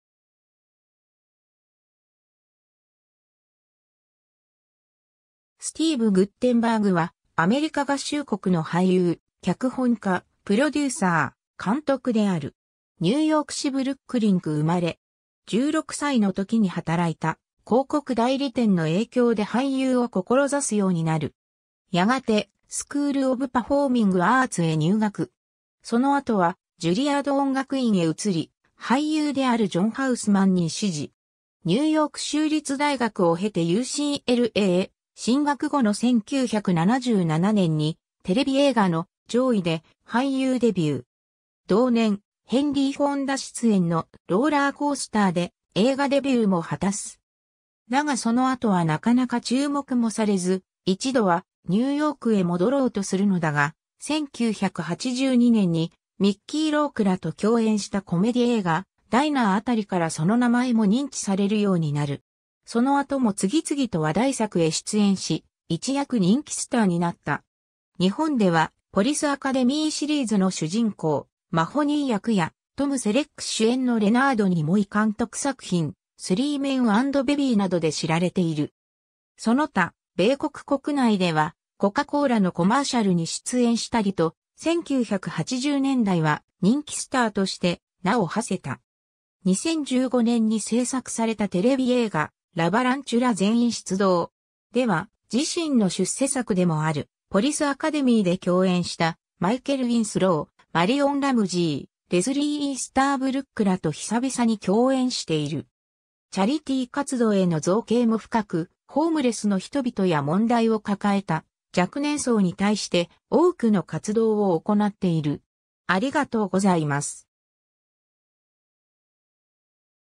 スティーヴ・グッテンバーグ
スティーヴ・グッテンバーグ（Steve Guttenberg、1958年8月24日 - ）は、アメリカ合衆国の俳優・脚本家・プロデューサー・監督である。
 ニューヨーク市ブルックリン区生まれ。
16歳の時に働いた広告代理店の影響で俳優を志すようになる。
やがてスクール・オブ・パフォーミング・アーツへ入学。
その後はジュリアード音楽院へ移り、俳優であるジョン・ハウスマンに師事。
ニューヨーク州立大学を経てUCLAへ進学後の1977年にテレビ映画の『ジョーイ』で俳優デビュー。
同年、ヘンリー・フォンダ出演の『Rollercoaster』で映画デビューも果たす。
 だがその後はなかなか注目もされず、一度はニューヨークへ戻ろうとするのだが、1982年にミッキー・ロークらと共演したコメディ映画『ダイナー』あたりからその名前も認知されるようになる。
その後も次々と話題作へ出演し、一躍人気スターになった。
 日本では『ポリスアカデミーシリーズ』の主人公マホニー役や、トム・セレック主演のレナード・ニモイ監督作品『スリーメン&ベビー』などで知られている。
その他、米国国内ではコカ・コーラのコマーシャルに出演したりと、1980年代は人気スターとして名を馳せた。
 2015年に製作されたテレビ映画『ラバランチュラ 全員出動!』では、自身の出世作でもある『ポリス・アカデミー』で共演したマイケル・ウィンスロー、マリオン・ラムジー、レスリー・イースターブルックらと久々に共演している。
 チャリティー活動への造詣も深く、ホームレスの人々や問題を抱えた若年層に対して多くの活動を行っている。